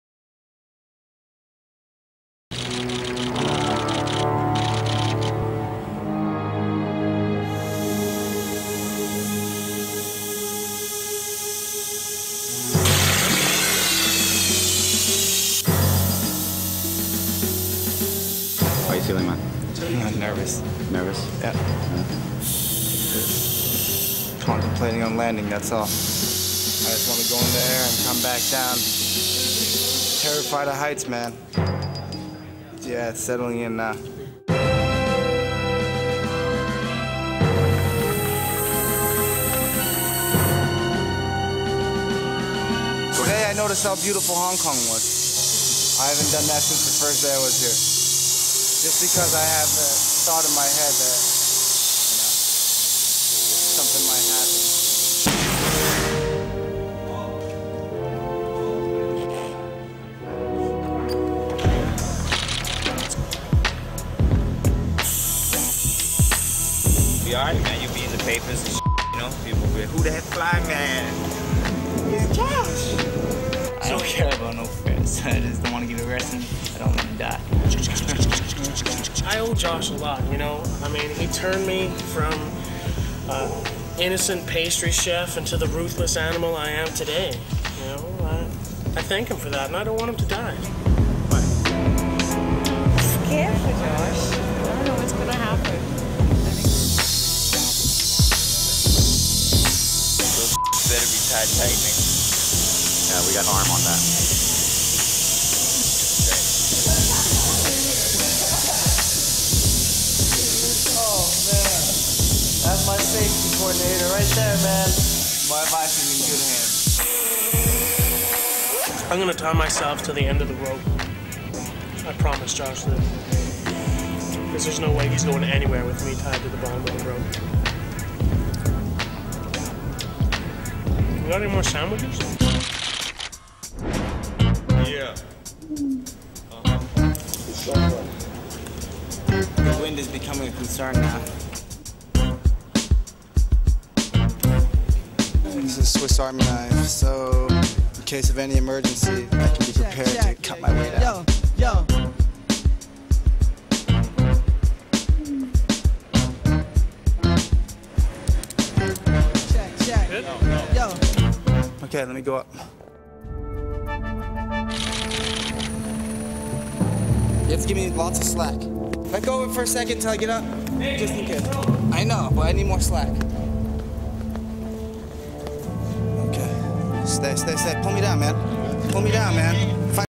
Balloon stunt over Victoria Harbour Hong Kong with Edison Chen
Watch as we take to the sky with our infamous balloon stunt over Victoria Harbour in Hong Kong !